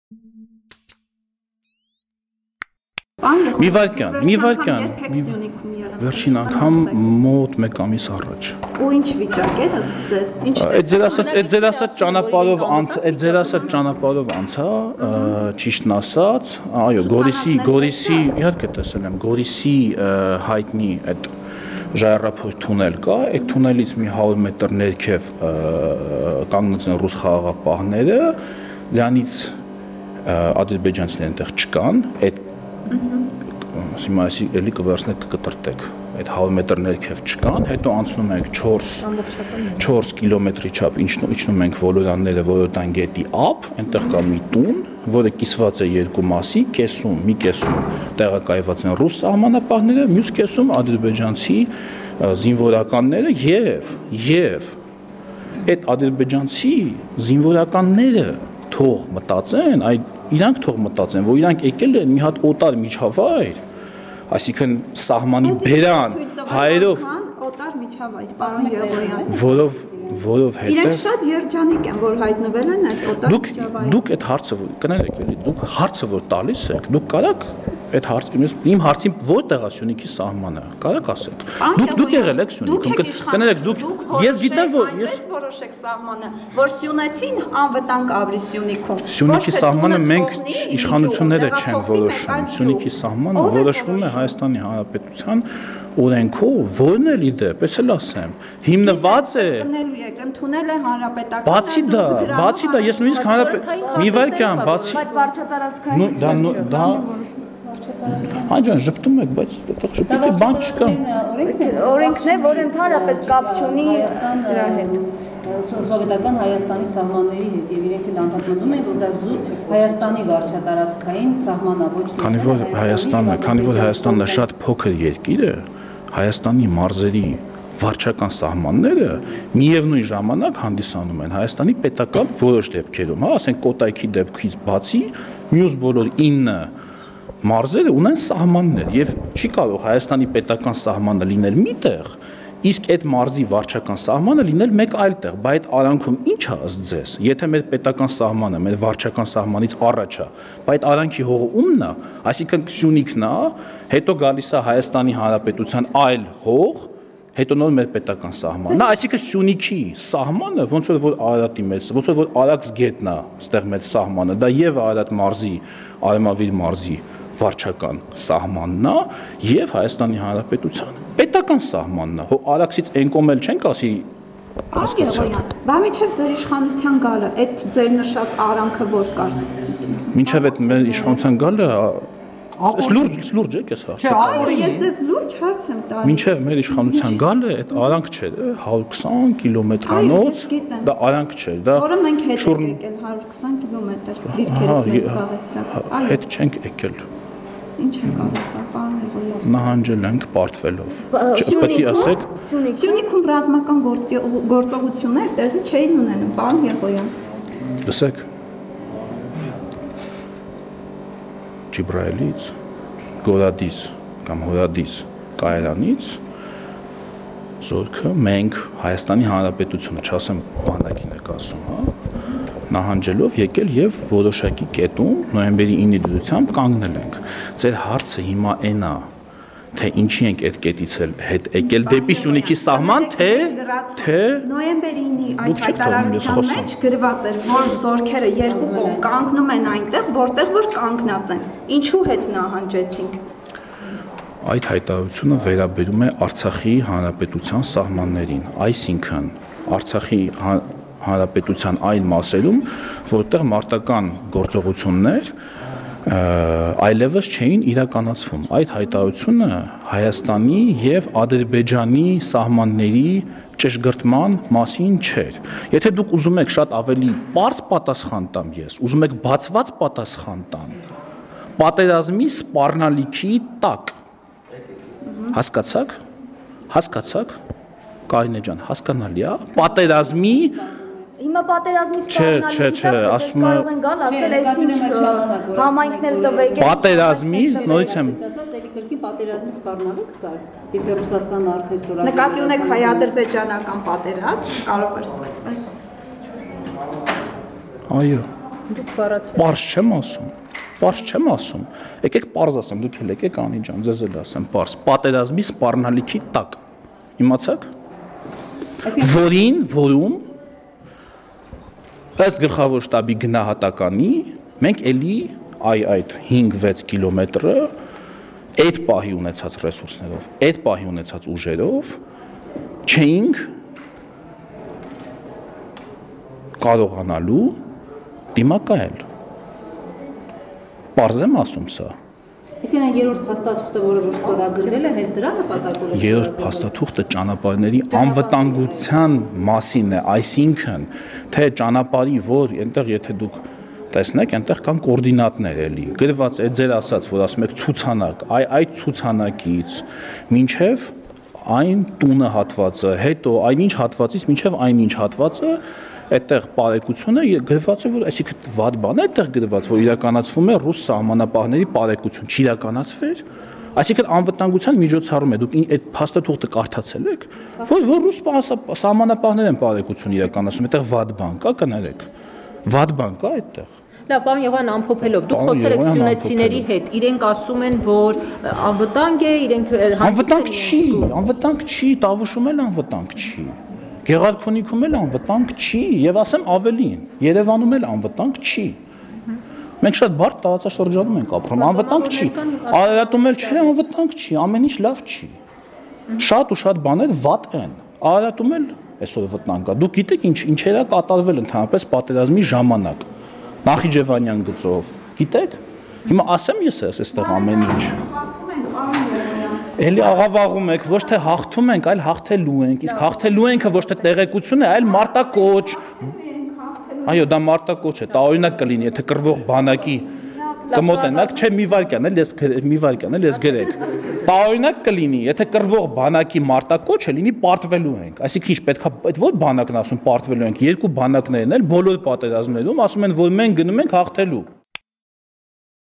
Սյունիքում տեղի ունեցածը պատերազմի սպառնալիքի տակ է եղել․ Արման Եղոյան
Ադրբեջանցի զինվորականները թող մտածեն, որ եկել են օտար միջավայր, սահմանի բերան․ իմքայլական պատգամավոր